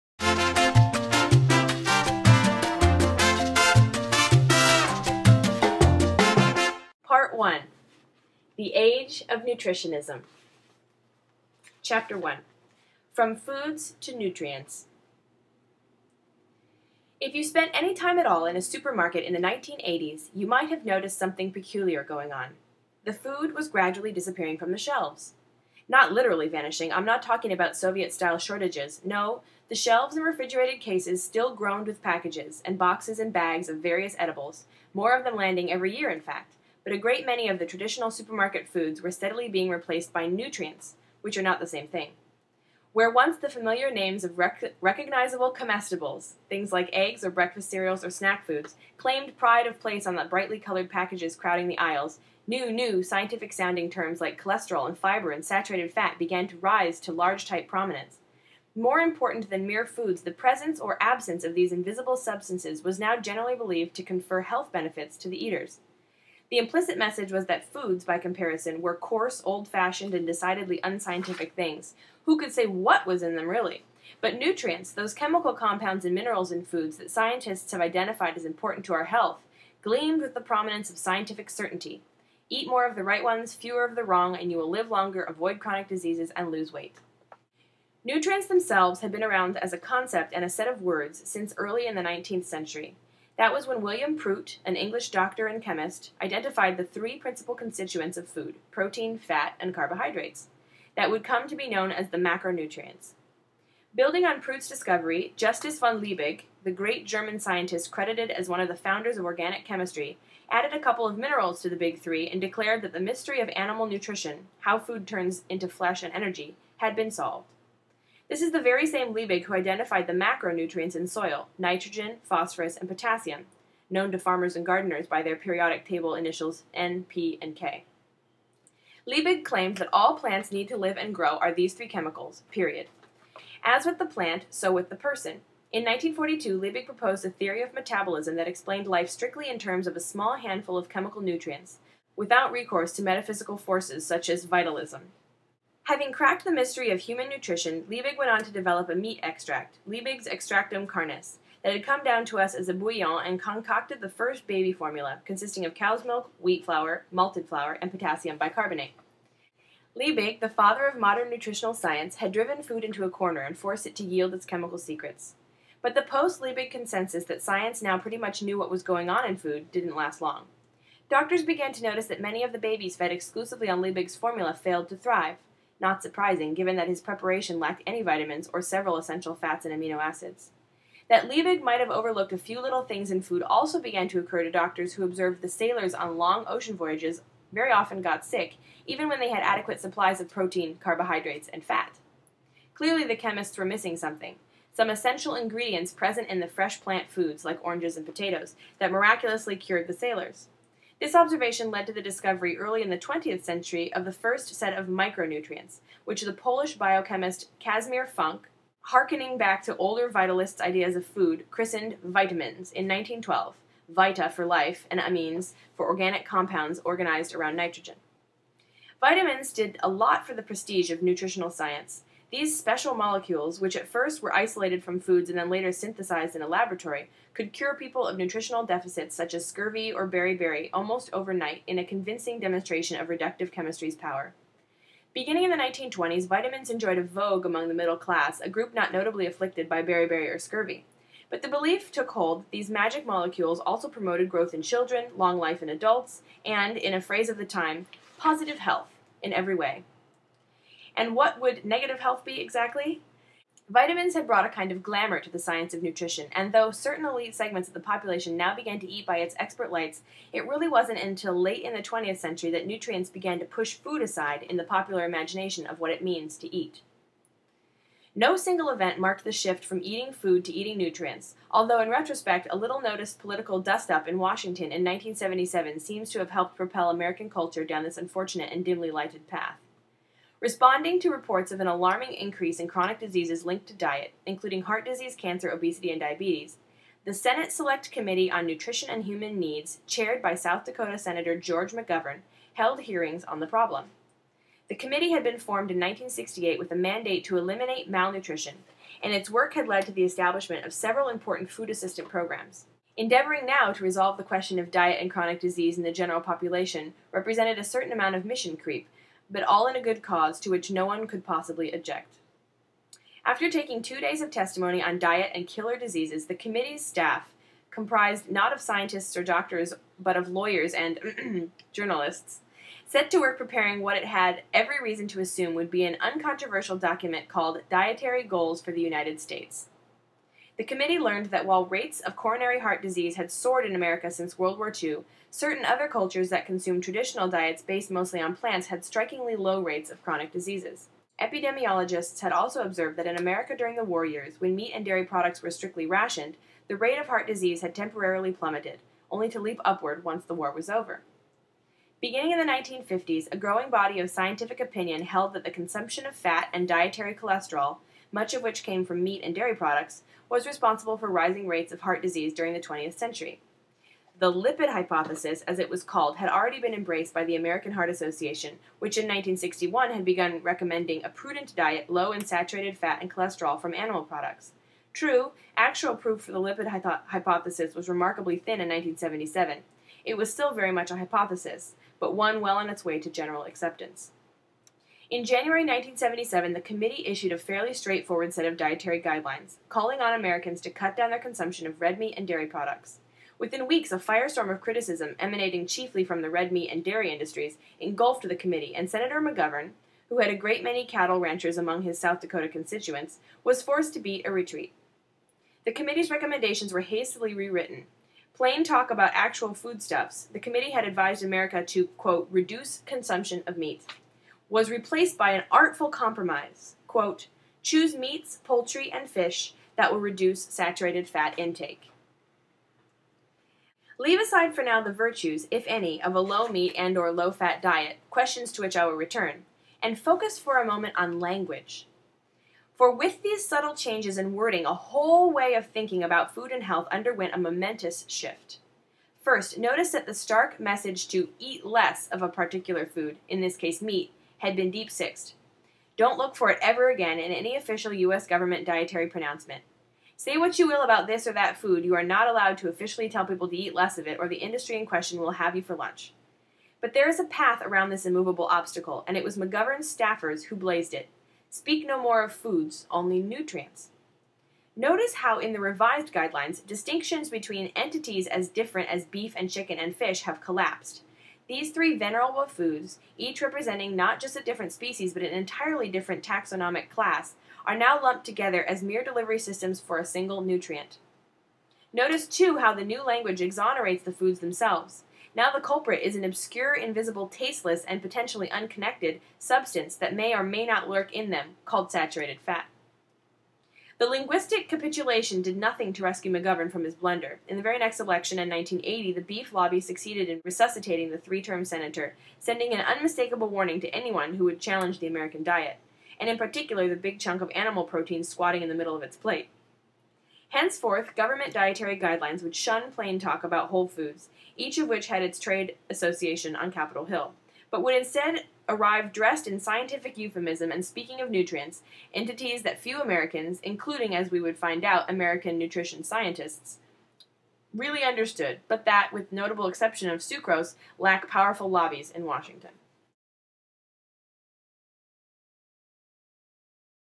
In Defense of Food 004 :: S1 Chapter 1 :: From Foods to Nutrients :: Part 1
This month we're reading In Defense of Food by Michael Pollan. Right now we're at:

Section: The Age Of Nutritionism.
Chapter: 1 - From Foods to Nutrients

This Video: this is the first part of chapter 1... part 2 coming later today!

Hope you're getting into the book!